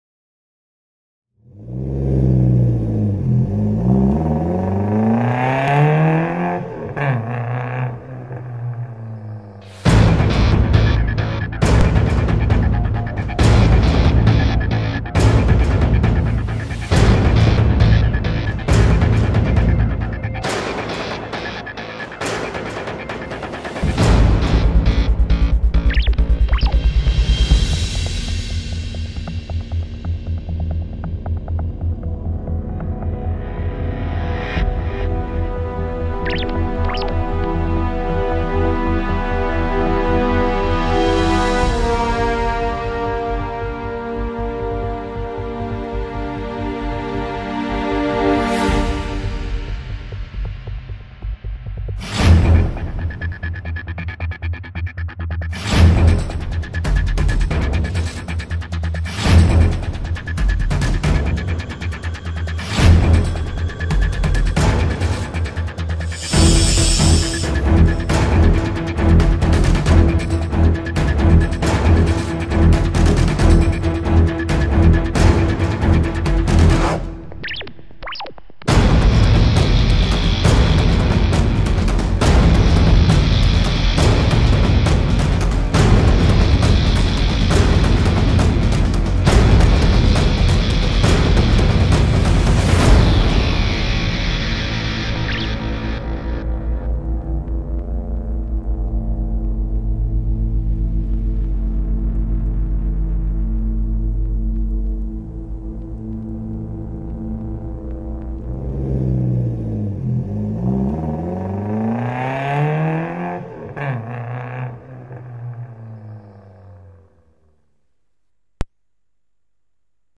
SHARK- work in progress Tedeschi Alessandro- Luca Zazzaretti Production
In attesa del video ufficiale, delle brevissime scene descriveranno appena la costruzione di un nuovissimo prototipo OFF-ROAD.
La T.A enginering di Tedeschi Alessandro è al lavoro per un progetto chiamato SHARK !
Buona visione !